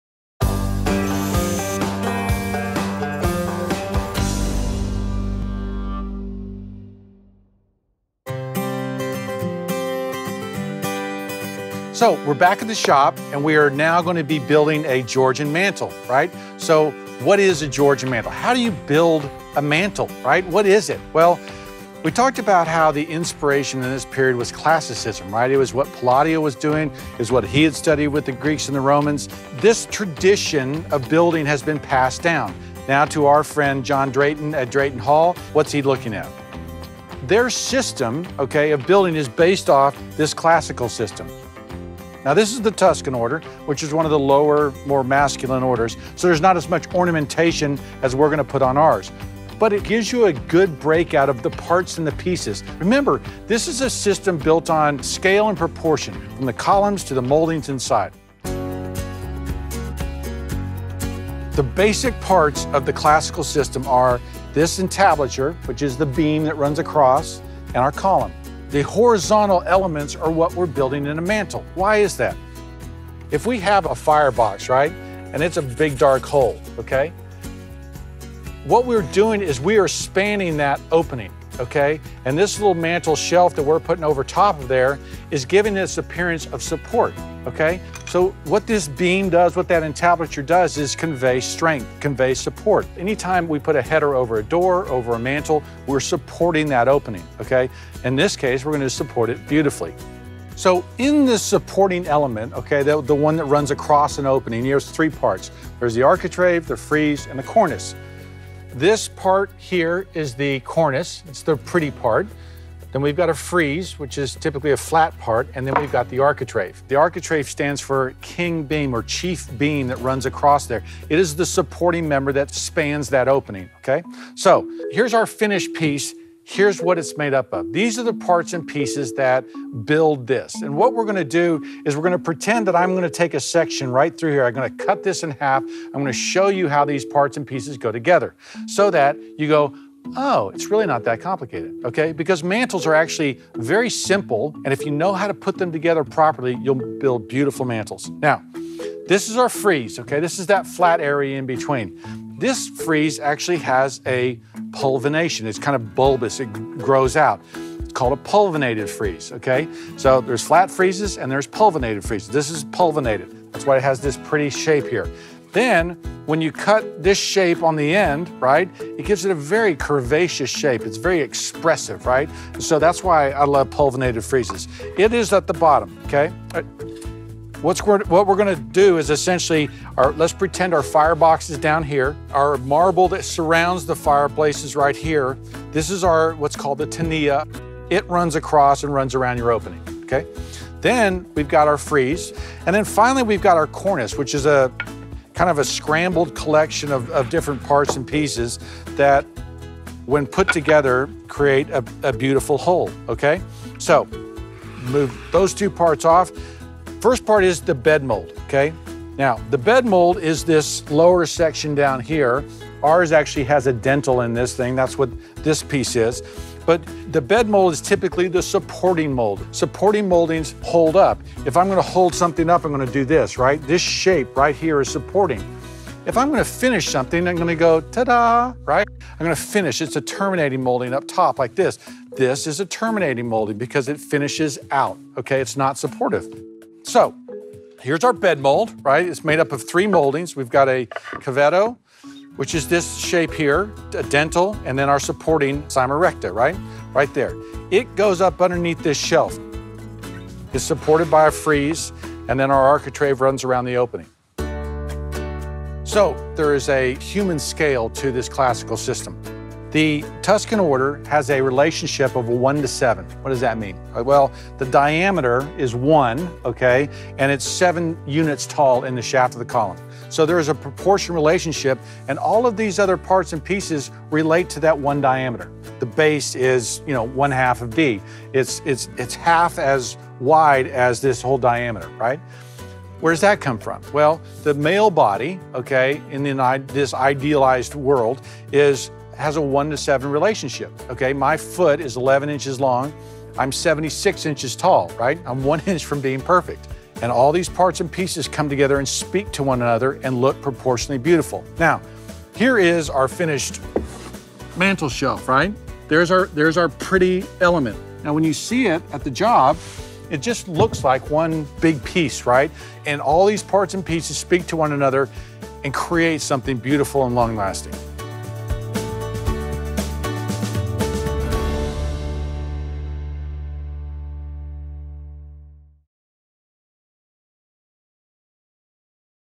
“Building with History” - Georgian Style - Workshop Edition
How To Build A Georgian Mantle

Last week, Brent introduced us to the Georgian Style of building. This week, he'll show us how to build a Georgian-style mantle. We'll look a the Tuscan Order, which does not have as much ornamentation as Brent will employ in his final piece, but gives us a good insight into the order and structure. Remember, this is a system built on scale and proportion.

The basic components are the beam that runs across and the column. The horizontal elements are what we're building in a mantle. Why? Because if we have a fire box, essentially a big dark hole, we're spanning that opening with a mantle that provides support. In this supporting element, we have three parts: the cornice, the frieze, and the architrave (the supporting element).

Learn more about the components of a Georgian mantle by watching the full video.

This kit library has links to books that will help you with classical and historical concepts and ideas. No extra cost to you.